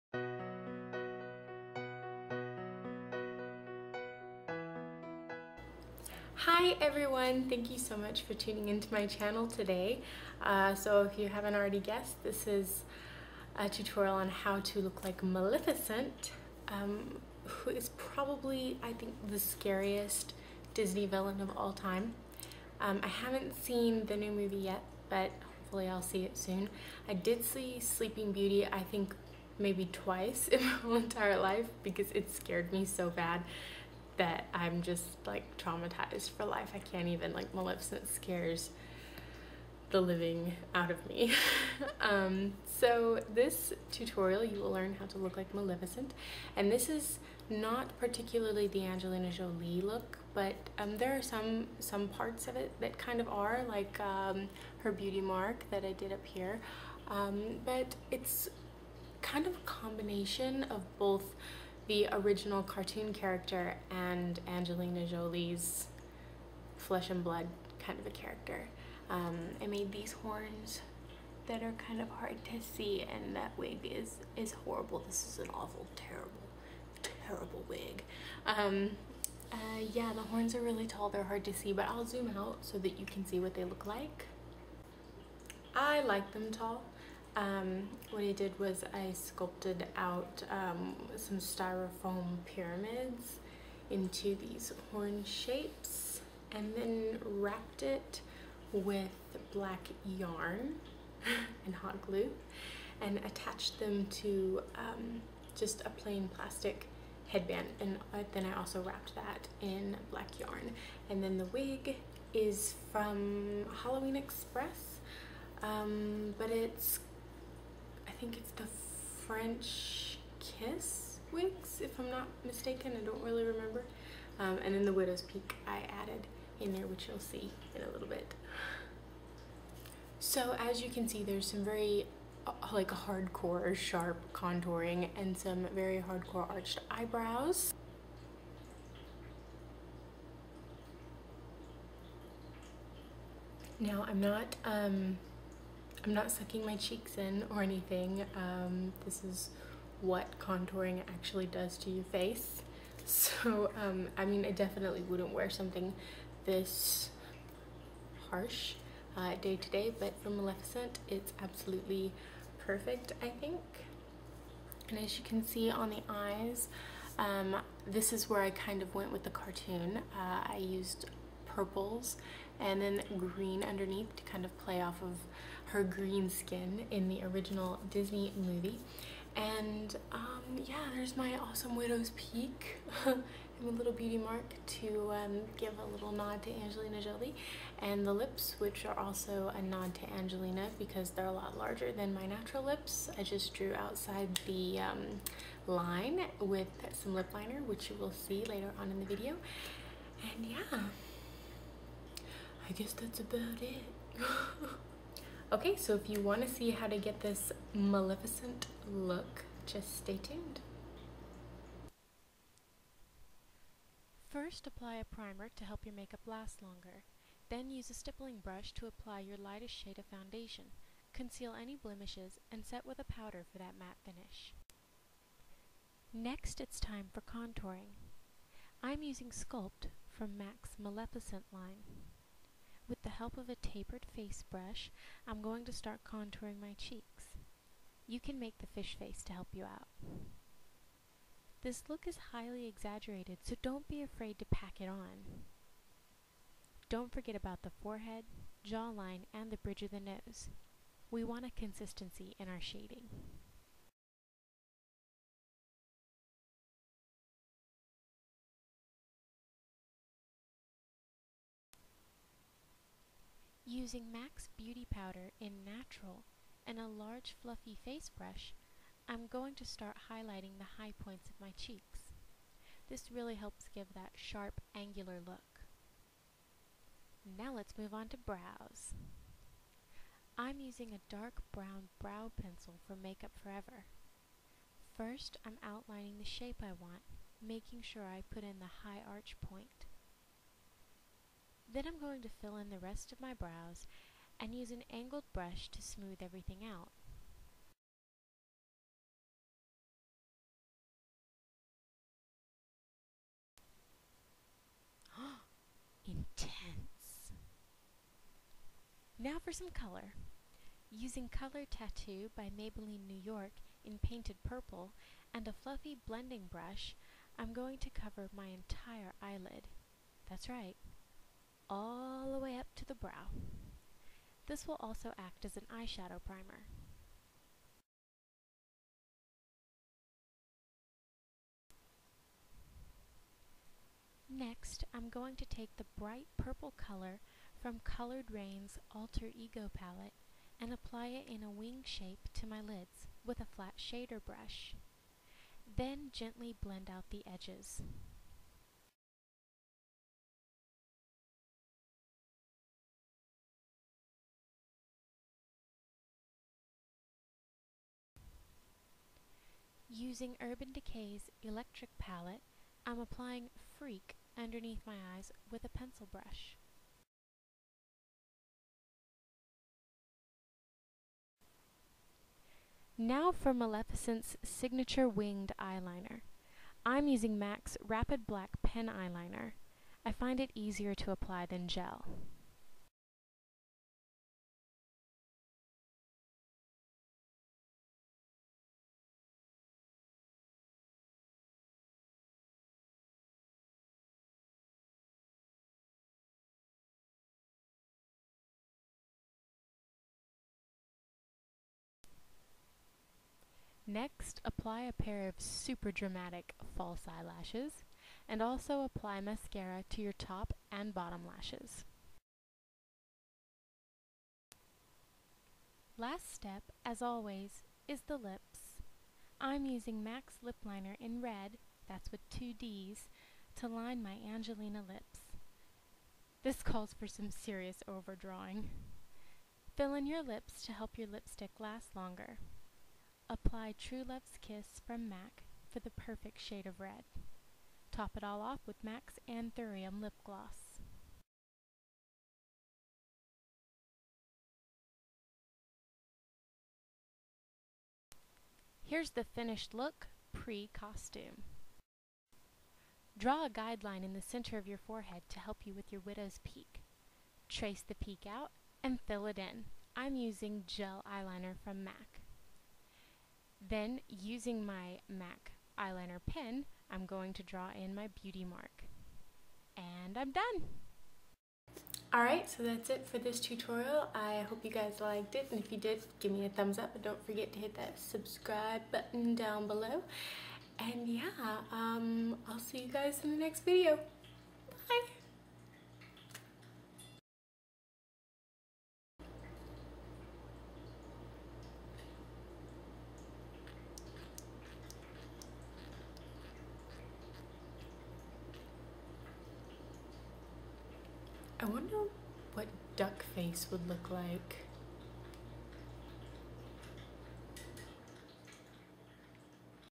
Maleficent Makeup Tutorial
Maleficent terrified me as a child, but I have managed to conquer my fears by becoming her.  I made the horns out of Styrofoam, hot glue, and yarn attached to a headband.  The wig was purchased at Halloween Express. It's the French Kiss Cheyenne wig.

The raven you see is not real.  It is a toy.

Products Used:
Becca Shimmering Skin Perfector (Opal)
MAC Studio Sculpt Foundation (NC15)
MAC Studio Sculpt Concealor (NC5)
MAC Contour Powder (Sculpt)
MAC Beauty Powder (Natural)
Makeup Forever Brow Pencil (No. 4)
Maybelline New York Color Tattoo (Painted Purple)
Coloured Raine Alter Ego Palette (bright purple)
Urban Decay Electric Palette (Freak)
MAC RapidBlack
MAC Lipliner (Redd)
MAC Lipstick (True Love's Kiss)
MAC Lipgloss (Anthurium)